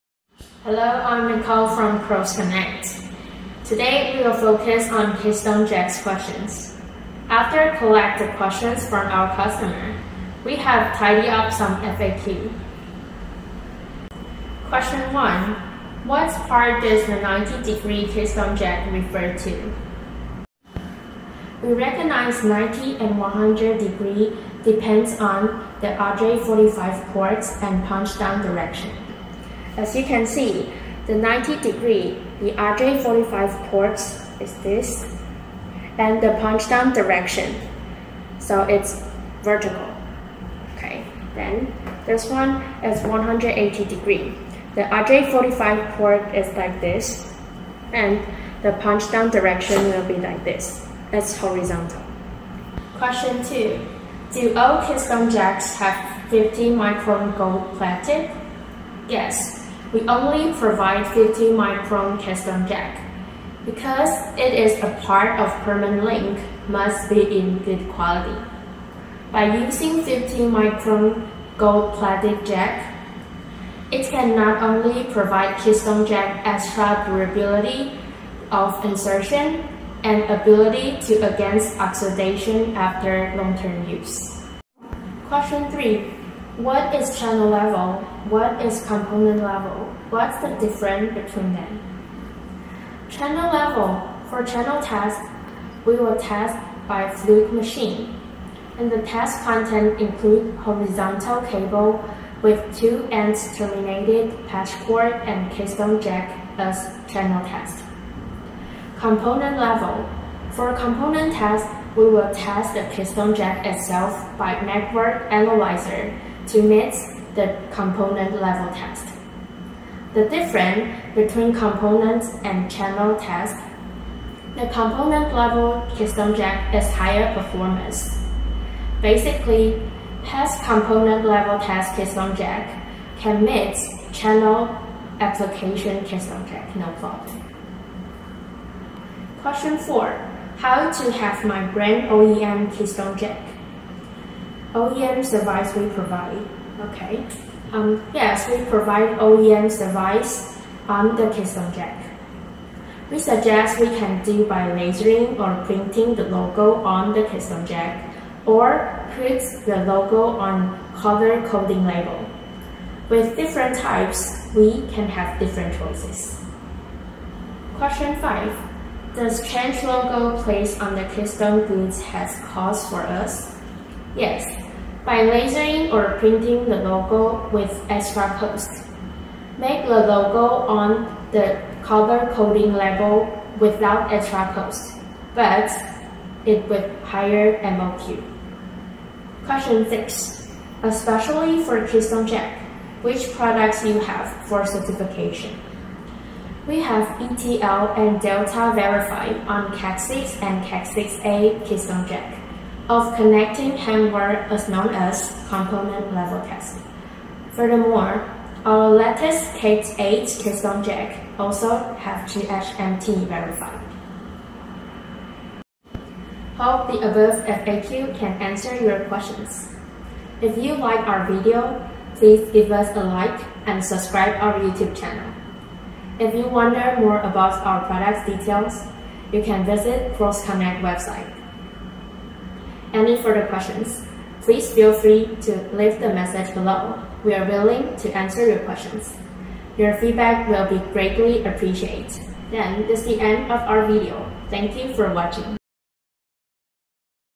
FAQ:6 Things about Keystone Jack Testing and OEM
Chapter
0:00 An intro of Q&A
0:15 What part does the 90-degree keystone jack refer to?
0:53 Do all keystone jacks have 50-micron gold plated?
1:26 What is Channel level, what is component level? What's the different between them?
2:19 How to have my brand OEM keystone jack? (oem service we provided)
2:48 Does change logo place on the keystone goods has cost for us?
3:10 Especially for keystone jack, which products you have for certification?